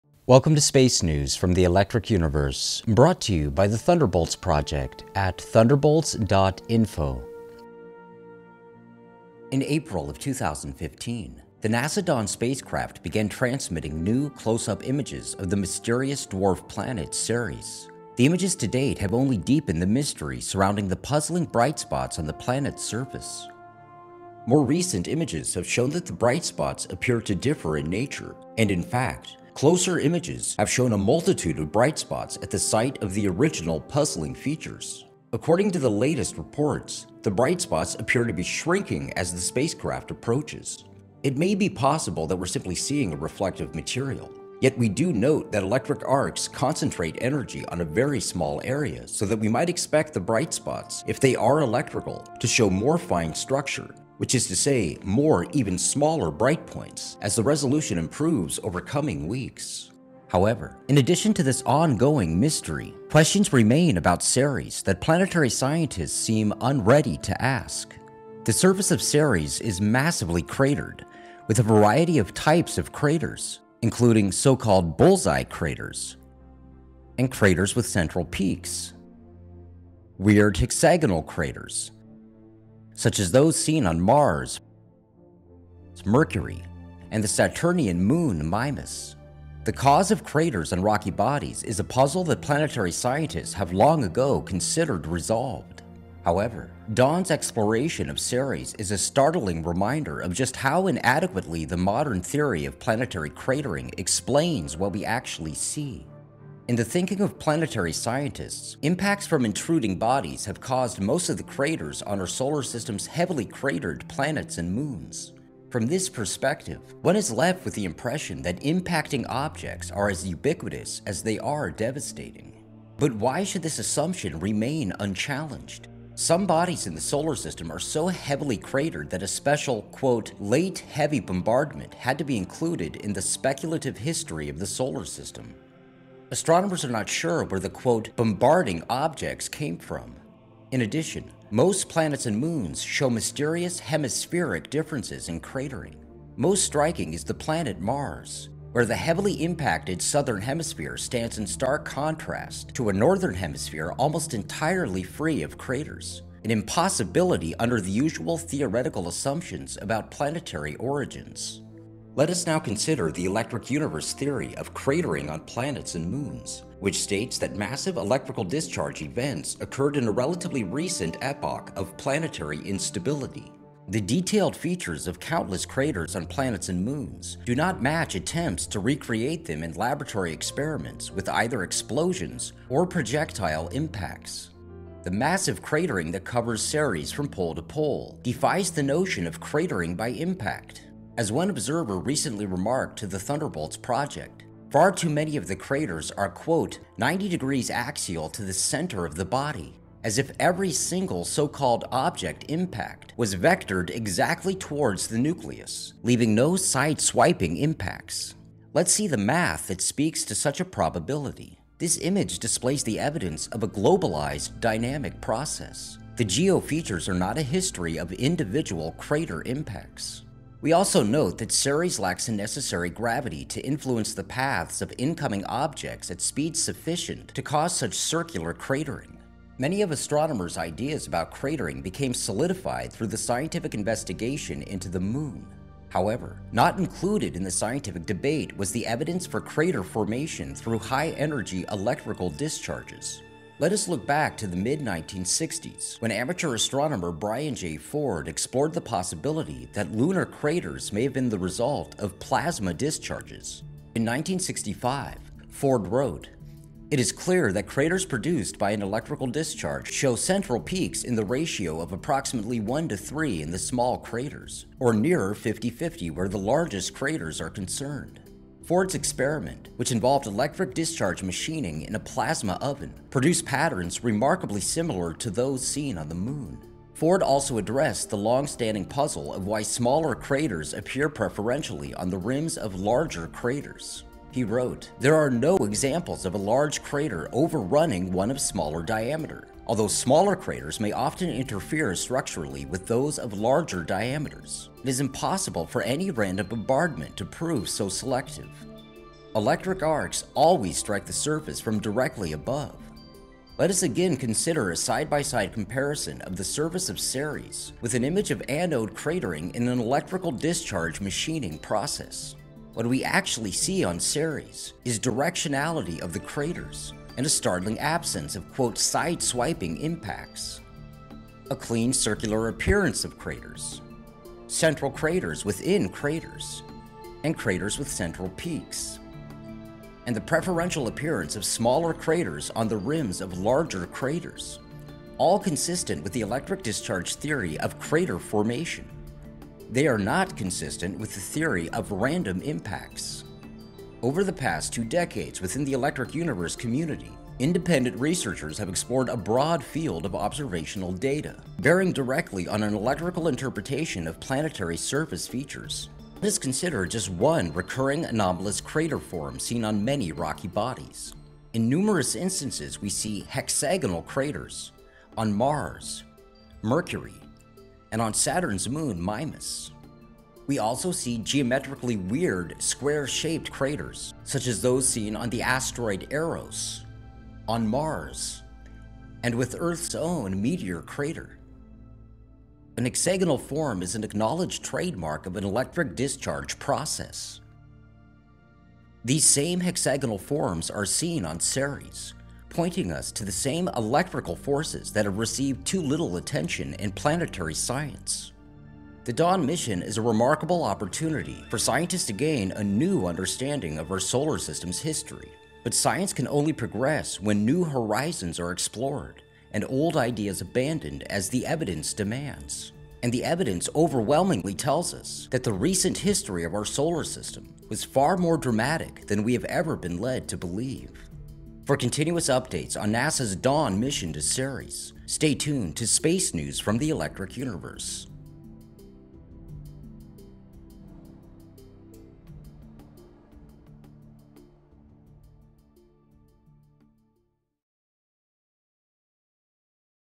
Ceres: An Electrically Scarred World | Space News
BUY YOUR TICKETS NOW for live global streaming of the full conference EU2015 or join us in person, June 25-29, Sheraton Phoenix Downtown.

In April of 2015, the NASA Dawn Spacecraft began transmitting new, close-up images of the mysterious dwarf planet Ceres. The images to date have only deepened the mystery surrounding the puzzling bright spots on the planet’s surface. However, in addition to this ongoing puzzle, questions remain about Ceres that planetary scientists seem unready to ask. The surface of Ceres is massively cratered, with a variety of types of craters, including so-called bulls-eye craters (craters with central craters), craters with central peaks, and weird hexagonal craters, such as those seen on Mars, Mercury, and the Saturnian moon Mimas. Dawn’s exploration of Ceres is a startling reminder of just how inadequately the modern theory of planetary cratering explains what we actually see on planets and moons.